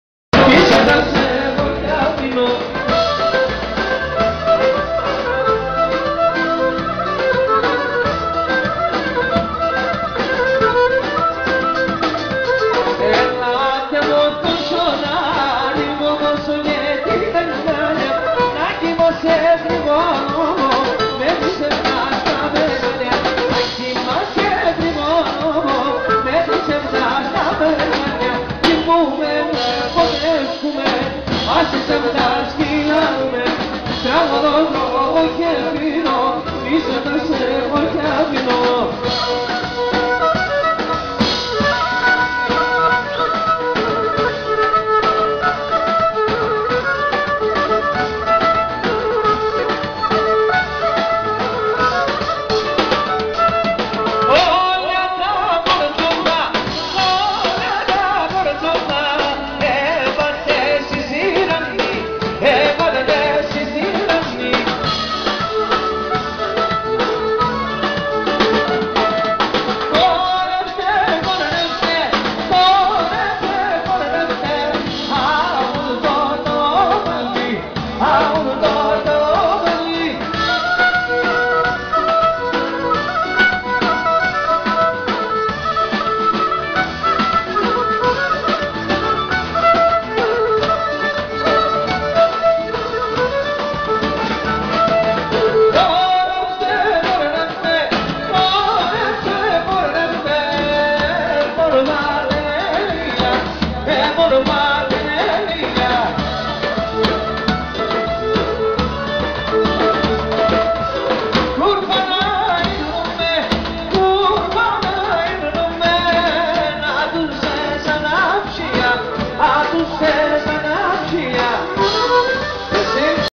SIAMLIDIS KOTIDIS MIMIS 05-01-2011 LAXANA
Γιώργος Σιαμλίδης,Θόδωρος Κοτίδης,Δημήτρης Φουντουκίδης,Γιώργος Κορτσινίδης,Κυριάκος Παπαδόπουλος και Δημήτρης Μιτρούλης στον ετήσιο χορό του συλλόγο ποντίων Ριζού στο κεντρο ΜΙΜΗΣ στις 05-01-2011.